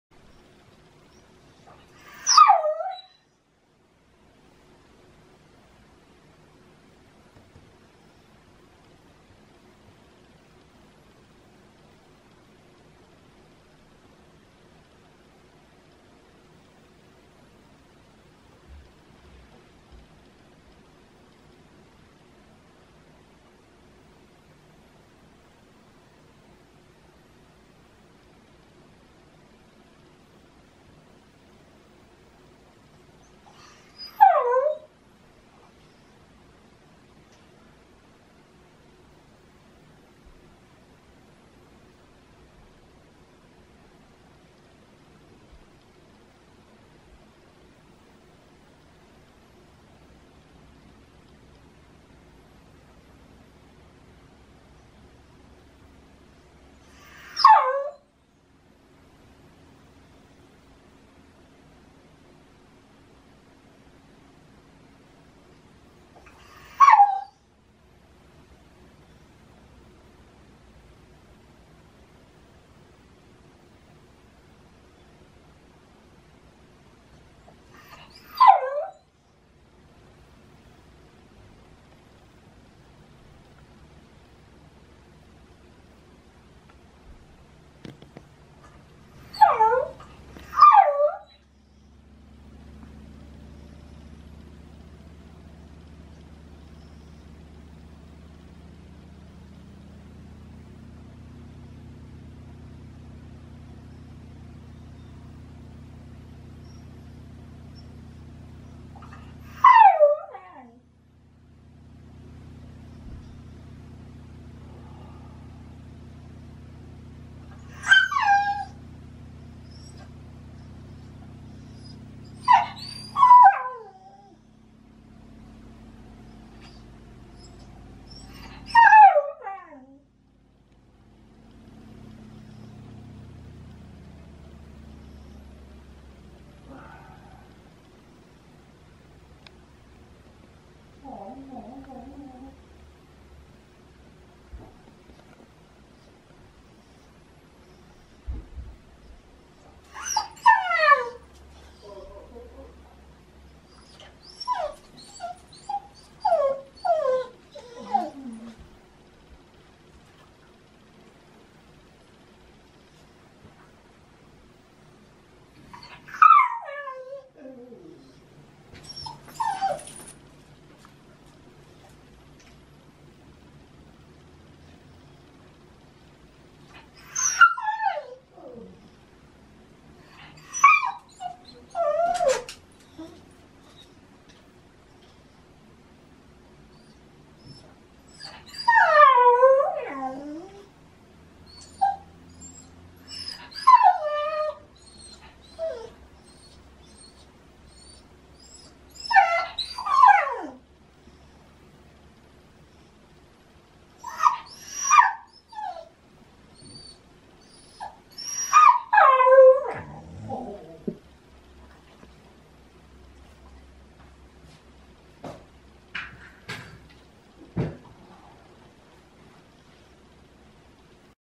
柴犬貘 恋の雄たけび⁉
この秋の恋の季節
朝早くから散歩の催促がうるさい？貘です。